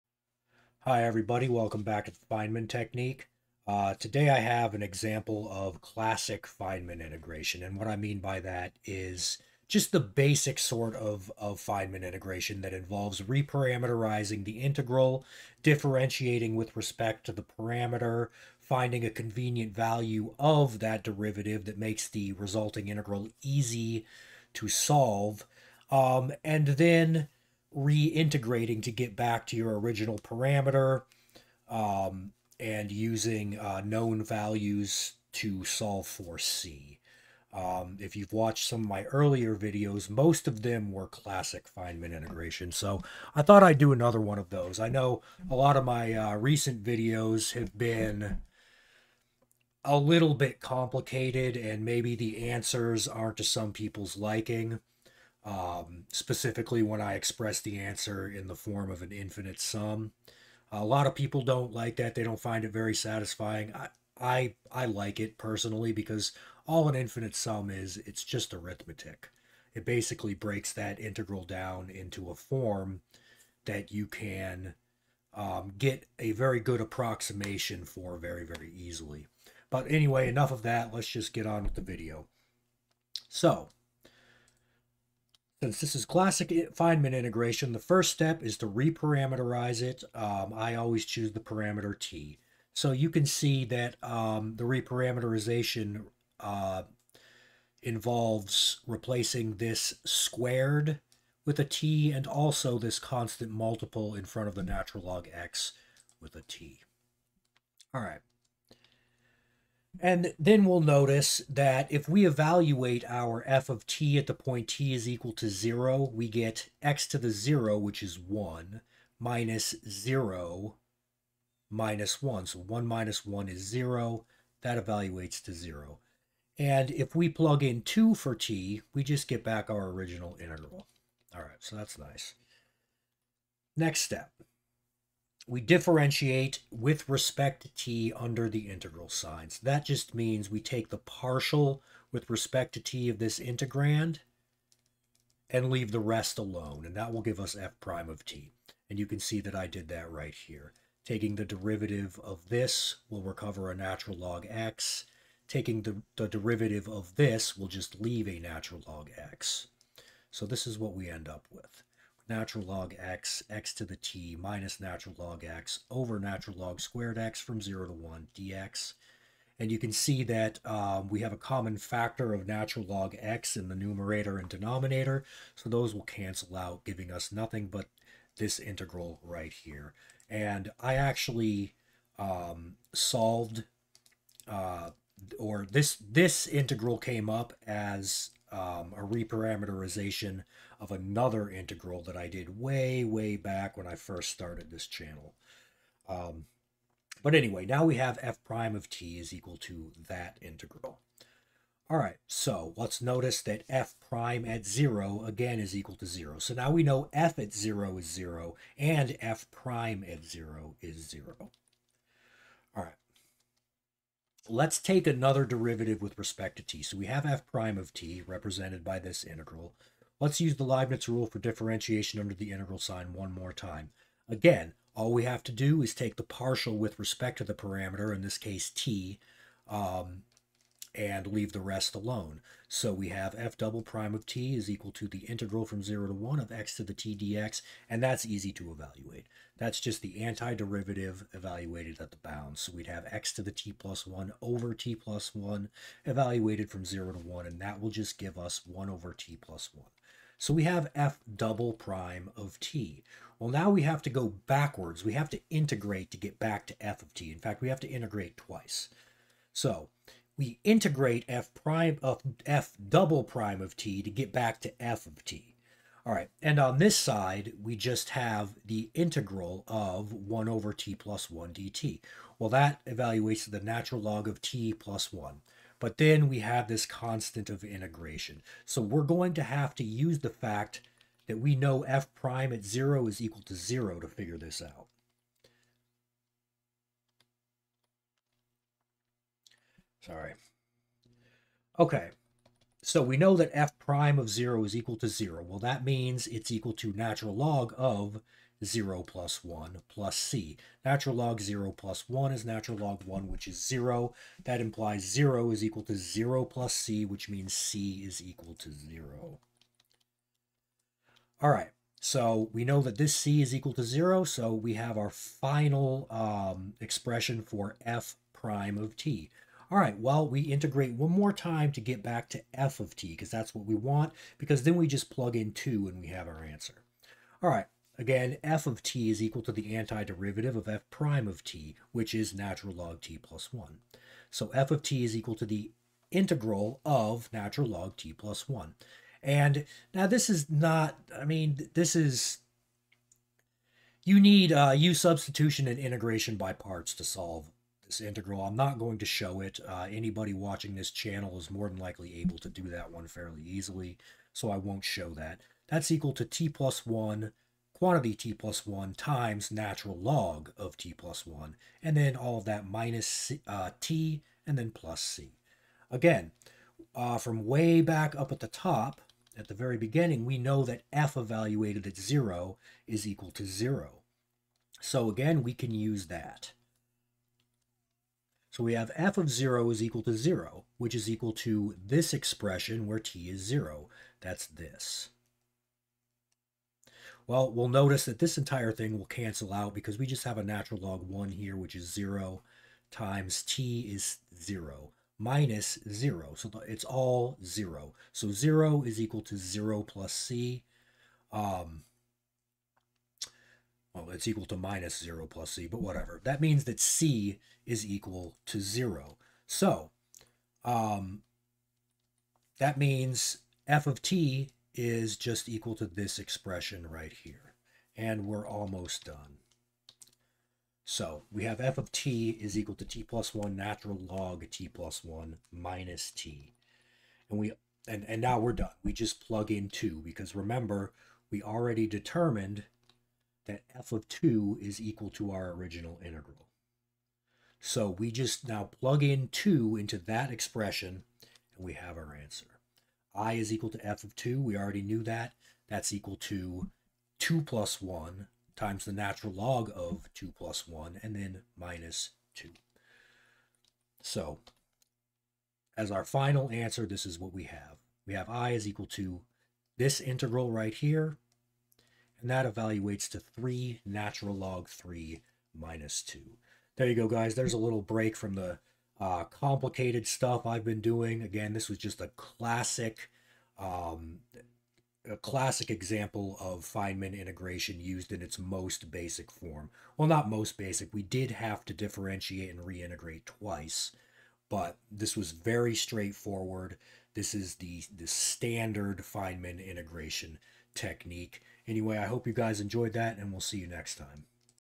Feynman Integration Example 131 - ∫ from 0 to 1 of ((x² - 2ln(x) - 1) / ln²(x)) dx
Evaluating ∫ from 0 to 1 of ((x² - 2ln(x) - 1) / ln²(x)) dx using Feynman integration.